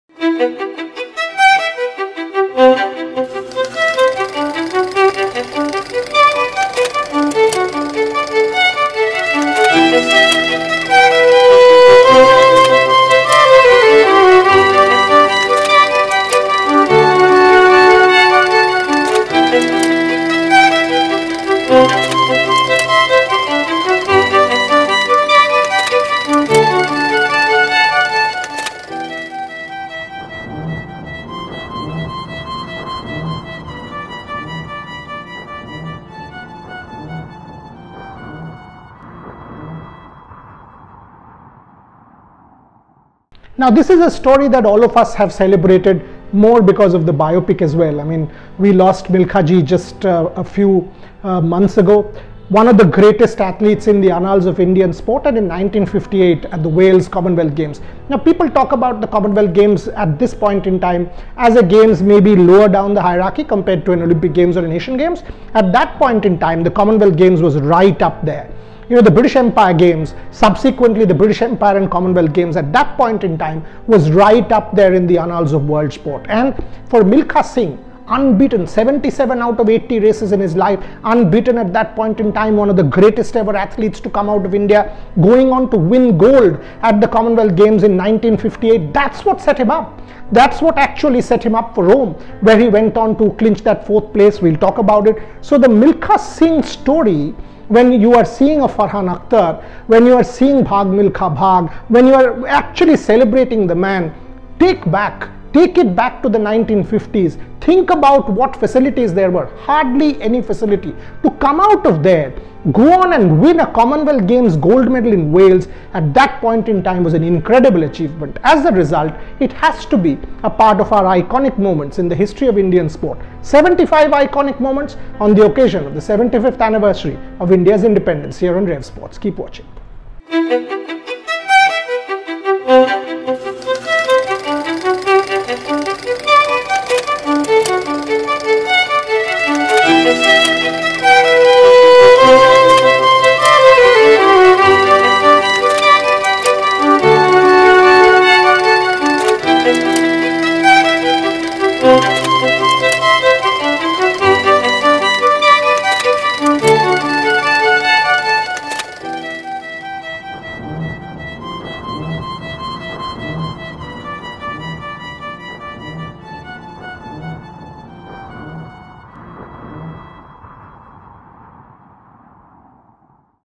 75 @ 75 || EP19 || CWG 1958, MILKHA SINGH
75 @ 75 EP19:
On the occasion of the 75th anniversary of Independence, we celebrate with 75 of our greatest sporting moments.
Winning 77 out of 80 races. 1958 CWG Gold 🥇. Celebrating